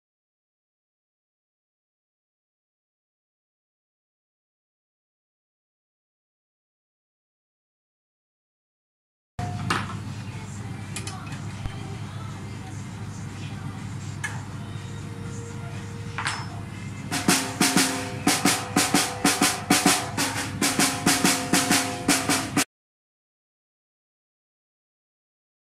LE SOPRANO PRESENT SNARE NEW VINTAGE CLASS 14"x 6,5"
LE SOPRANO SNARE NEW VINTAGE CLASS 14"x 6,5"
Rullanti per veri intenditori. Per chi desidera che il suono del proprio strumento sia la sintesi del passato con tutta la tecnologia moderna.
Rullanti per un’applicazione JAZZ e/o CLASSICA.
La definizione di questo rullante ne fa un eccezionale mezzo per rivivere le atmosfere calde e di massima sensibilità.

Snare drums for true connoisseurs. For those who want the sound of their instrument to be the synthesis of the past with all modern technology.
Snare drums for a JAZZ and / or CLASSIC application.
The definition of this snare drum makes it an exceptional means to relive warm atmospheres and maximum sensitivity.

Tambores para verdaderos conocedores. Para aquellos que quieren que el sonido de su instrumento sea la síntesis del pasado con toda la tecnología moderna.
Tambores para una aplicación JAZZ y / o CLASSIC.
La definición de este tambor lo convierte en un medio excepcional para revivir ambientes cálidos y la máxima sensibilidad.

Video by Nirmala Gambirasio

The video channel of Le Soprano:
Telephone Number: 035/551063
Sito Web: